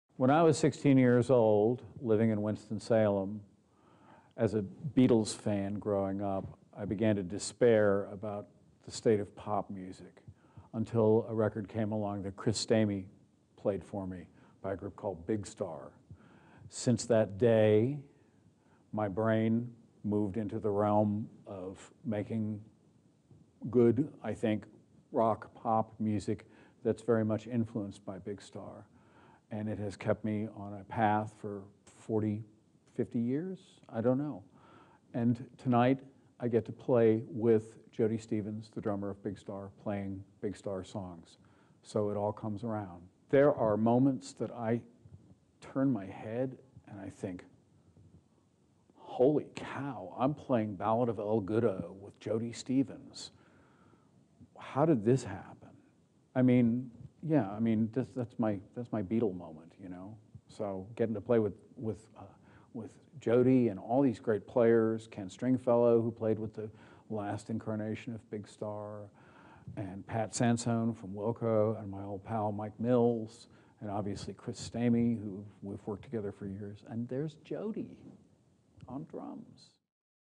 Peter Holsapple, “How Big Star Saved Pop Rock”
Peter Holsapple describes his dismay as a Beatles fan growing up in Winston-Salem, North Carolina when subsequent pop rock, in his opinion, failed to live up to their high standard. That all changed one day when he listened to an album by the band Big Star. He reminisces about how the opportunity to play with the band’s drummer in a tribute concert has brought his musical journey full circle.

Peter Holsapple is a musician, originally from North Carolina, who is a founding member of the dB’s. He has also played with Rittenhouse Square, R.E.M., and Hootie & the Blowfish.